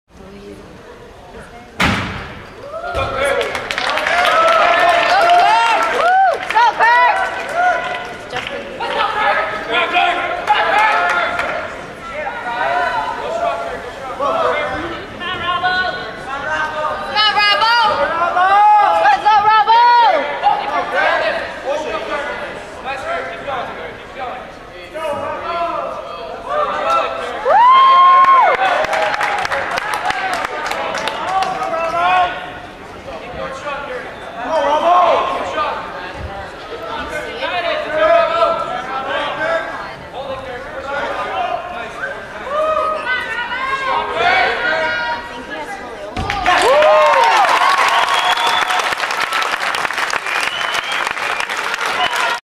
Rings vs Naperville North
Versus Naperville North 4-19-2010

Rings:  9.0   (1st)
Floor X:  9.7   (1st)
Pommels:   8.6   (1st)
Vault:   9.1   (1st)
PBars:   9.3   (1st)
HiBar:   9.4   (1st)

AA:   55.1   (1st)